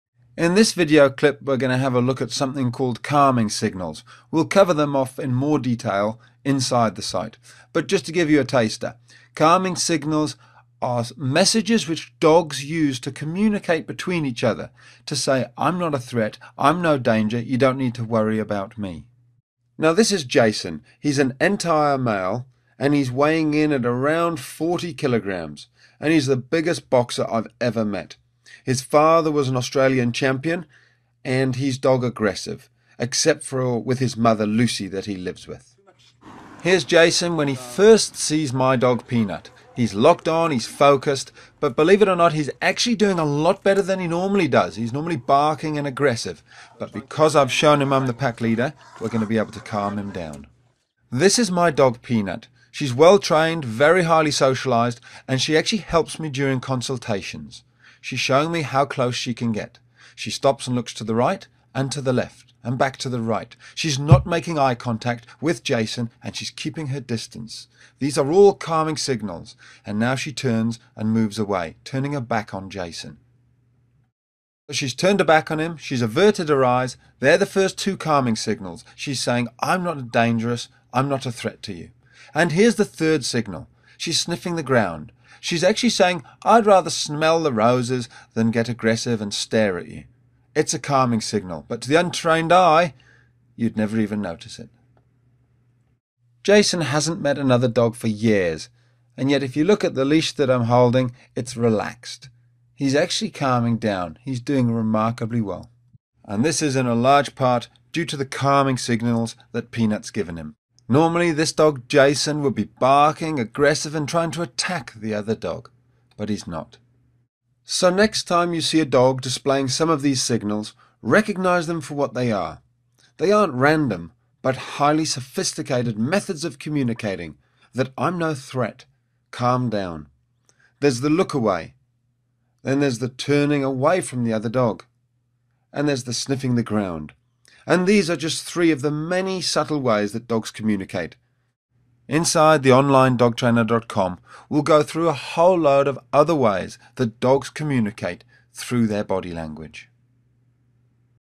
Avoid Dog Aggression by Recognizing Calming Signals
Watch Dog expert Doggy Dan calm down Jason a massive aggressive Boxer by using his own dog Peanut to demonstrate Calming Signals in Jason's presence. Excellent quick tutorial on Dog Body Language and Communication